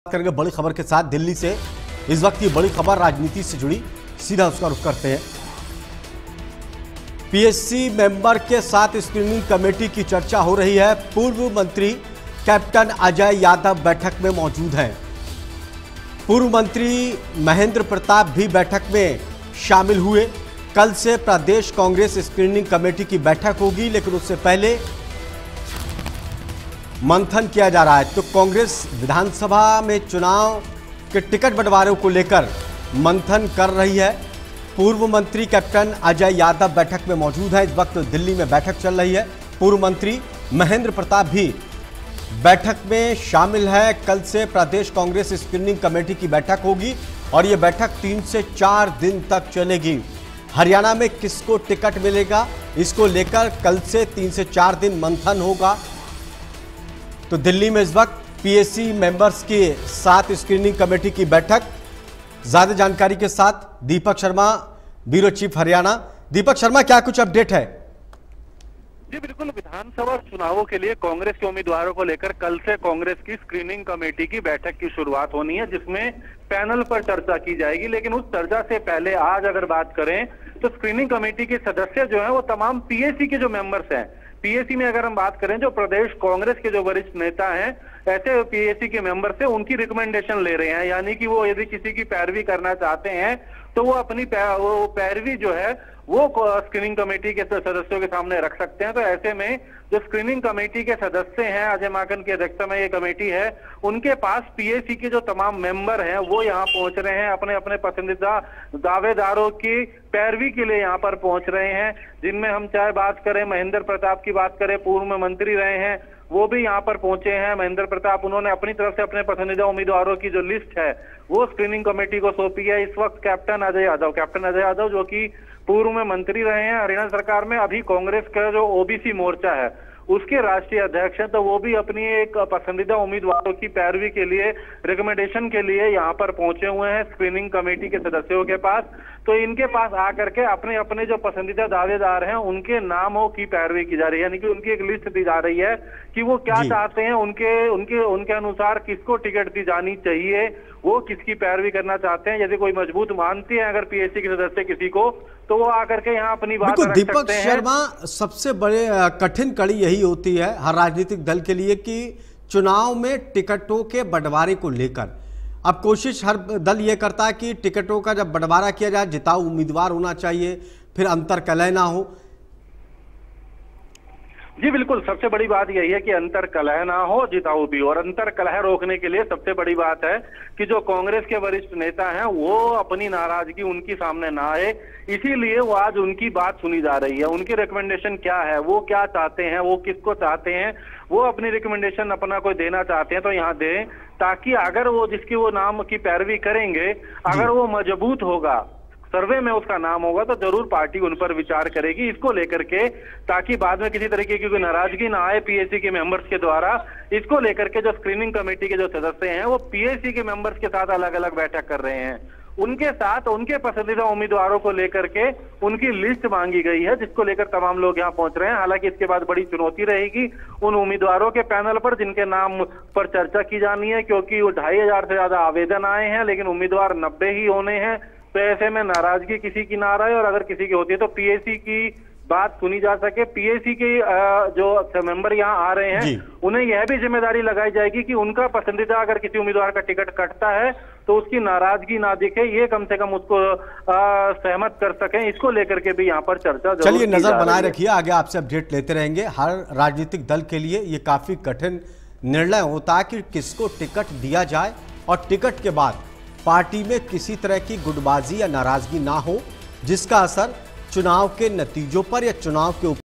Haryana Vidhansabha Chunav में टिकट को लेकर Cong स्क्रीनिंग कमेटी बैठक में पार्टी ने लिया बड़ा फैसला!
हरियाणा विधानसभा चुनाव में टिकट बंटवारे को लेकर आज दिल्ली में  PAC मेंबर्स के साथ कांग्रेस स्क्रीनिंग कमेटी की चर्चा हुई ,जिसमे पूर्व केंद्रीय मंत्री कैप्टन अजय यादव बैठक में मौजूद थे साथ ही पूर्व मंत्री महेंद्र प्रताप भी बैठक में शामिल हुए। जहाँ यह बैठक आगामी 3 से 4 दिन चलेगी स्क्रीनिंग कमेटी की बैठक, इस बैठक में टिकट के दावेदारों के नाम पर होगी चर्चा! India News Haryana
Congress Screening Committee discussed with PAC members in Delhi today regarding ticket distribution in Haryana Assembly elections, in which former Union Minister Captain Ajay Yadav was present in the meeting and former Minister Mahendra Pratap also attended the meeting. While this meeting will continue for the next 3 to 4 days, the screening committee meeting will be held and the names of the ticket contenders will be discussed in this meeting.